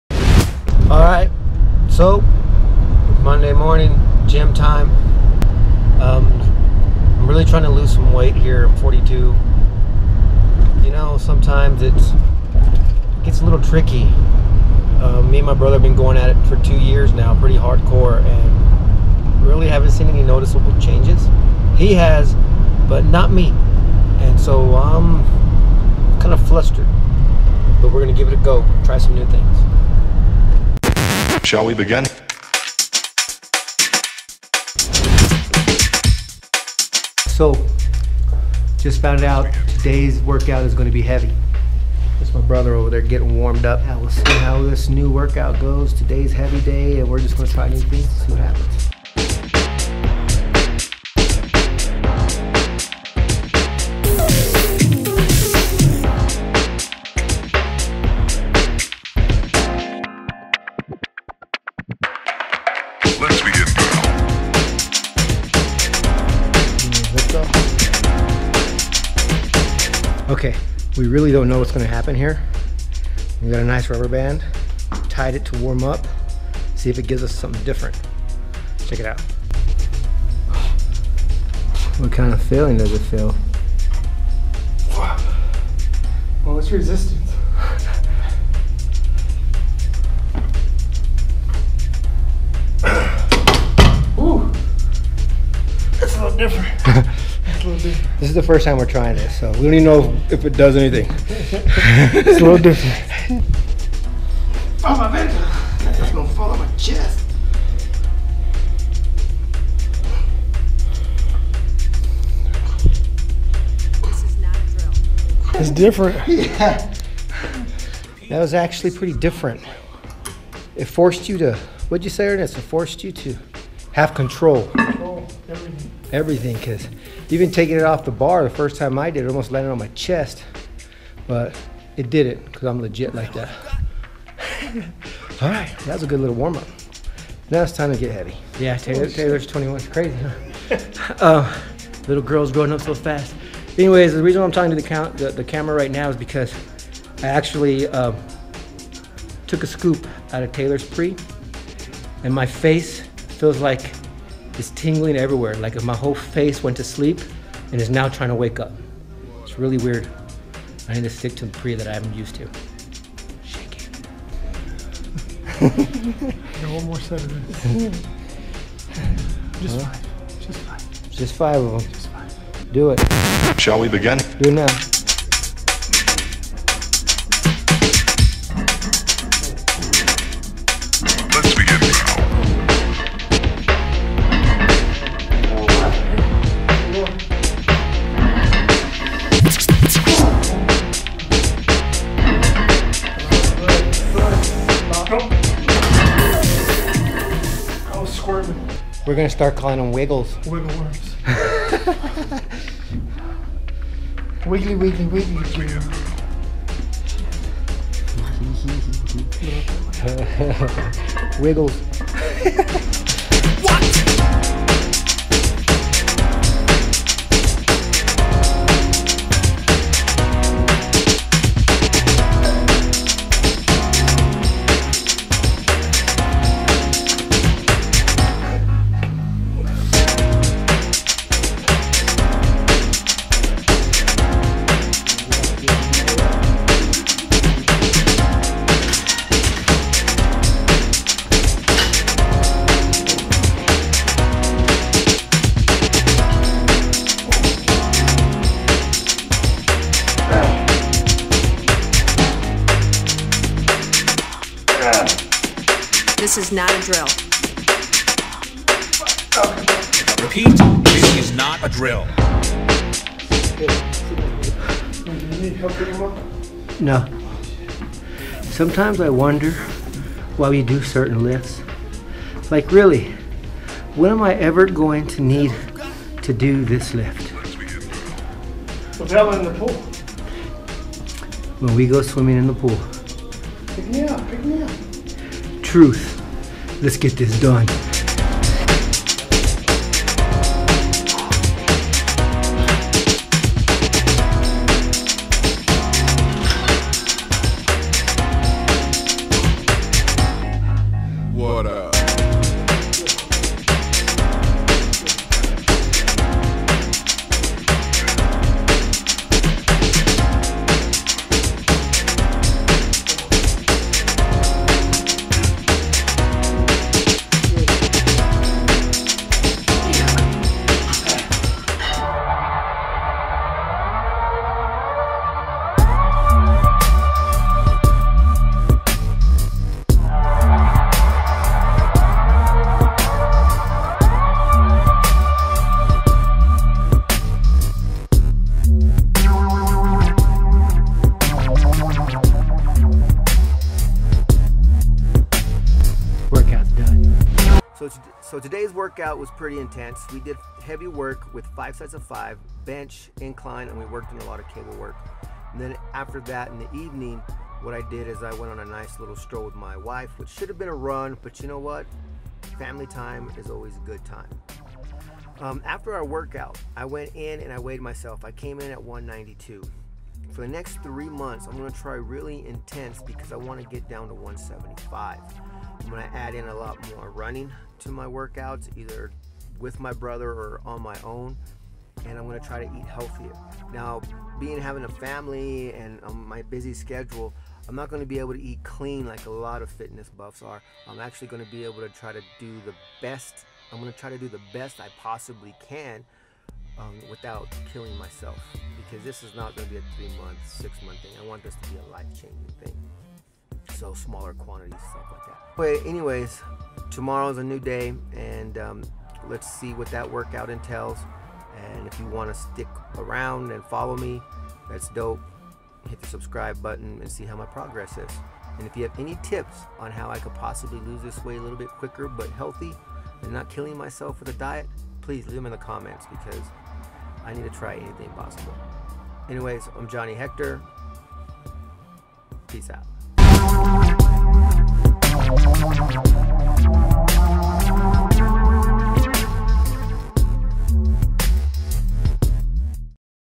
Fitness Goals
Day one of 12 weeks to try and get to 170. My brother is my trainer...